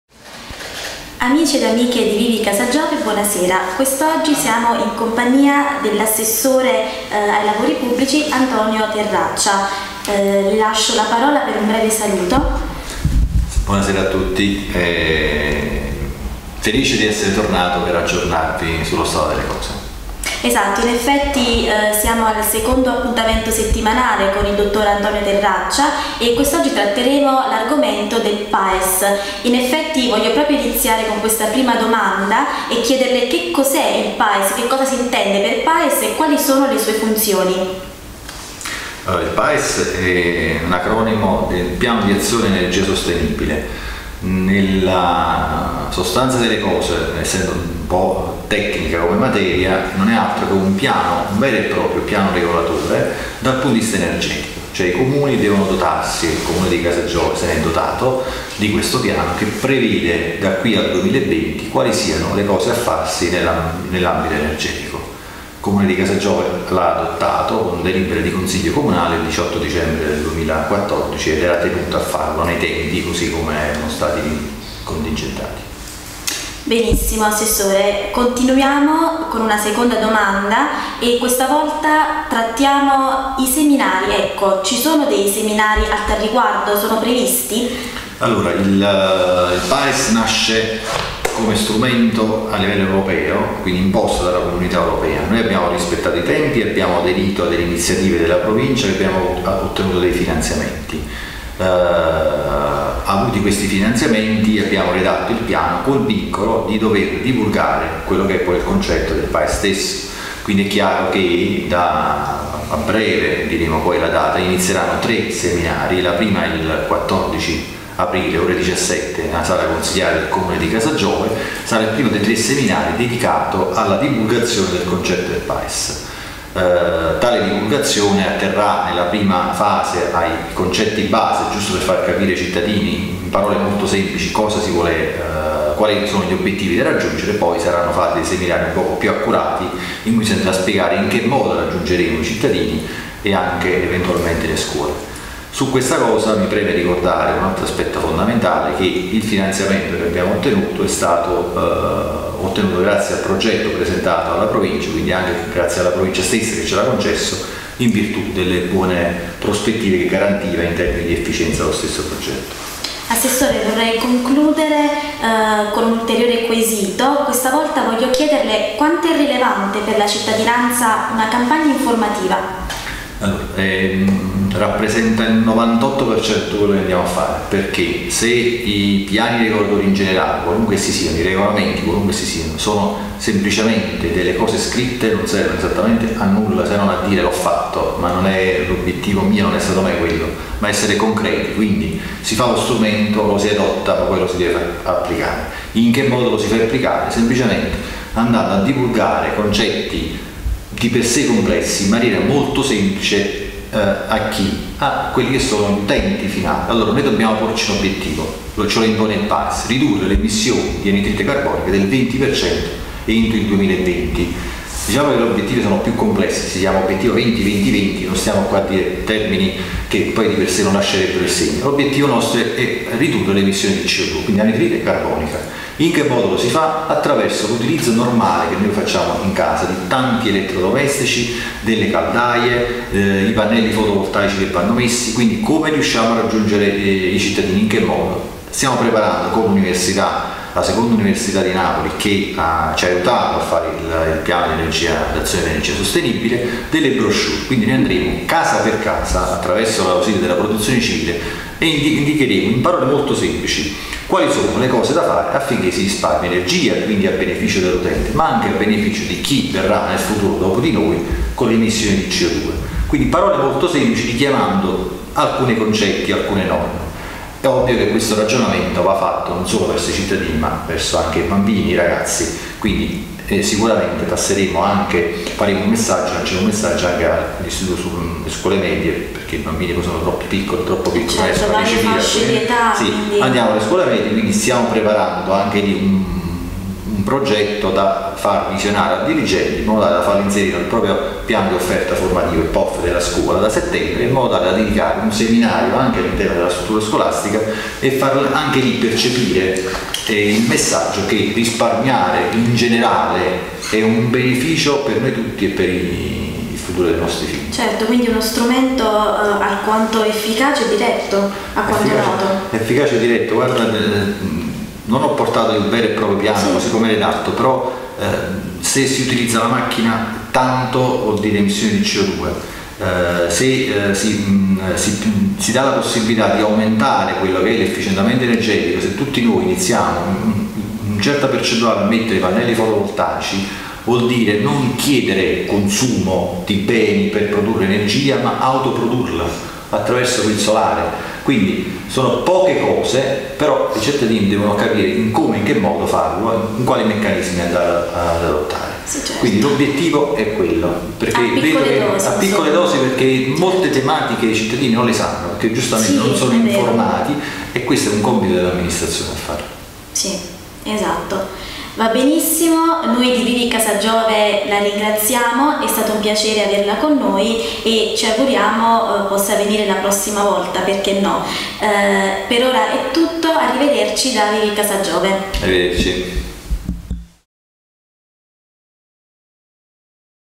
Intervista assessore Terraccia a cura di Michela Lagnena
Intervista assessore Terraccia a cura di Michela Lagbena. Riprese e montaggio a cura di Francesco Avezzano.